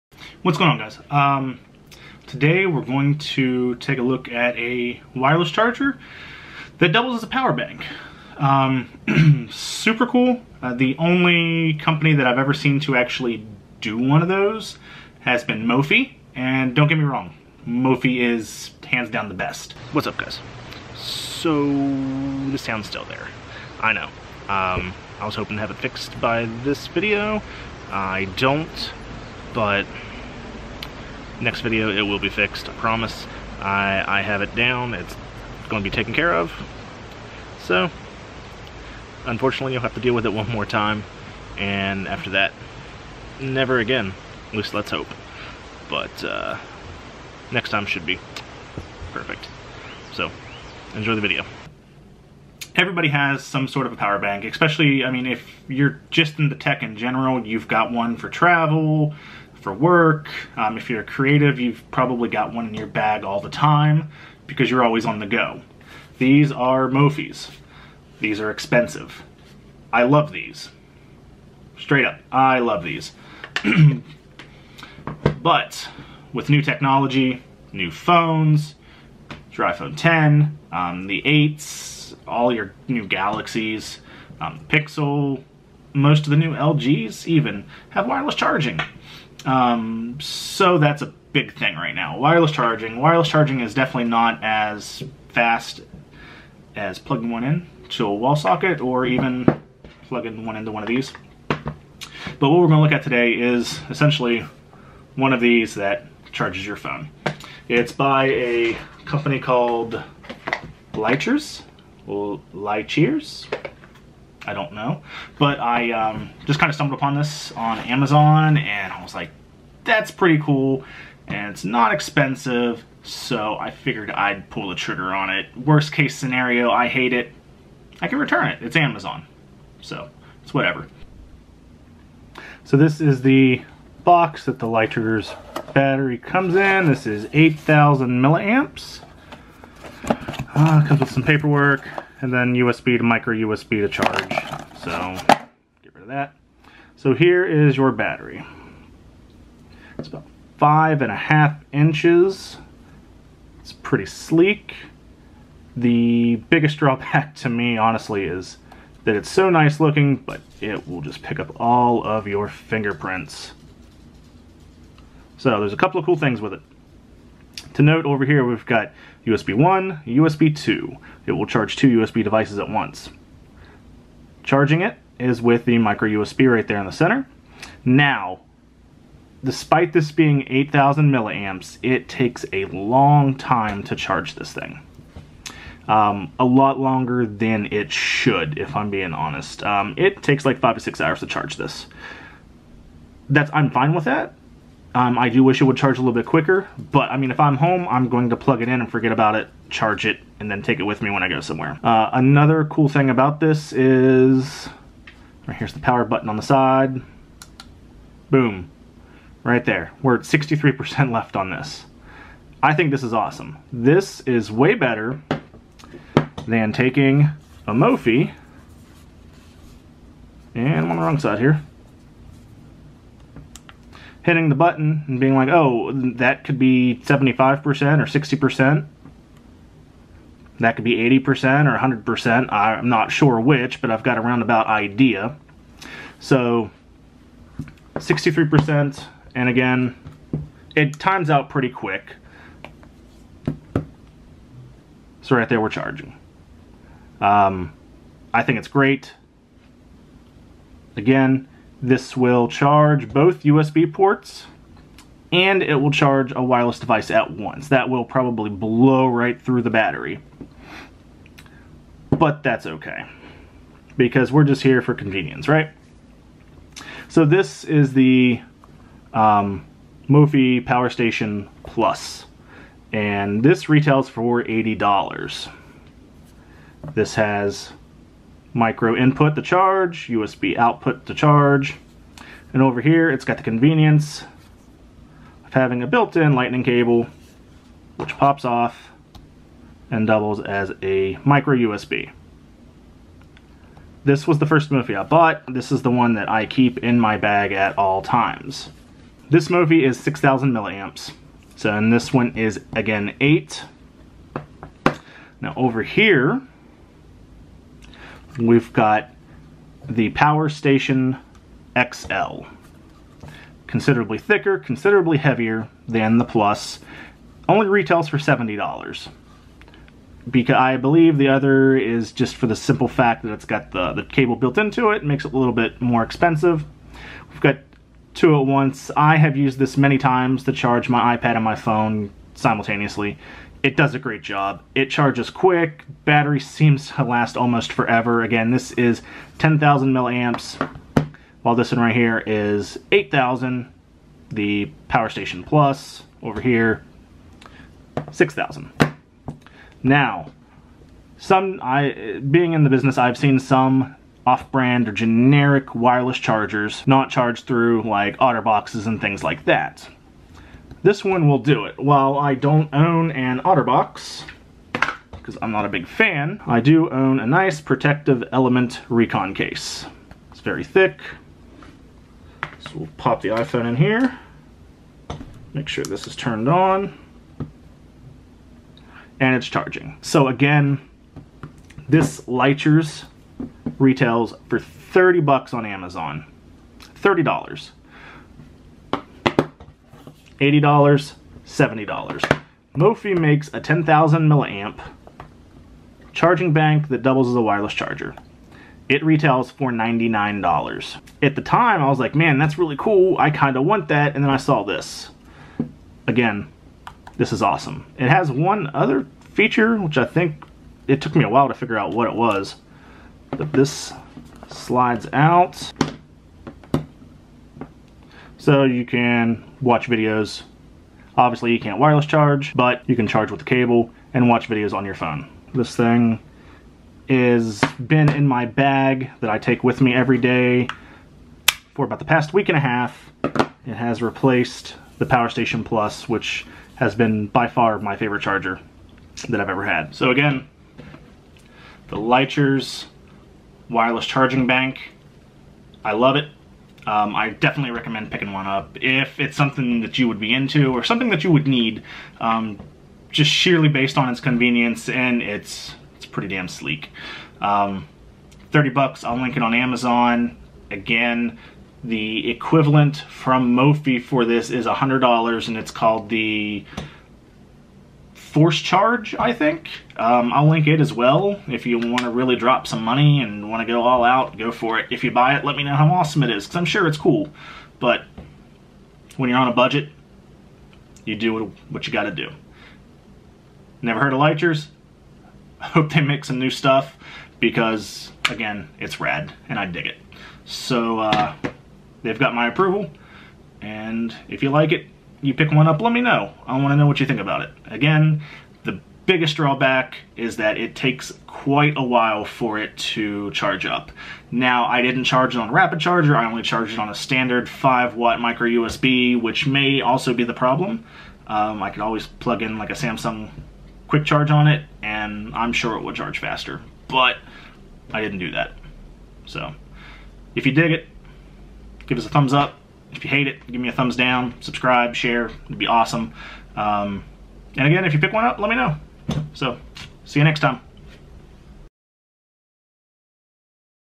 A Power Bank That Doubles As A Wireless Charger?
Review for the Licheers wireless charging power bank.
So the sound is still a little wonky, but I will def be able to fix it. Next video, the sound will be gone. Promise. haha.

Licheers Power Bank
Mophie Powerstation XL
Mophie Powerstation Plus
Mophie Charge Force

My Gear:
Smart Keyboard
Mini Tripod 1
Mini Tripod 2: h
iPhone Camera Lenses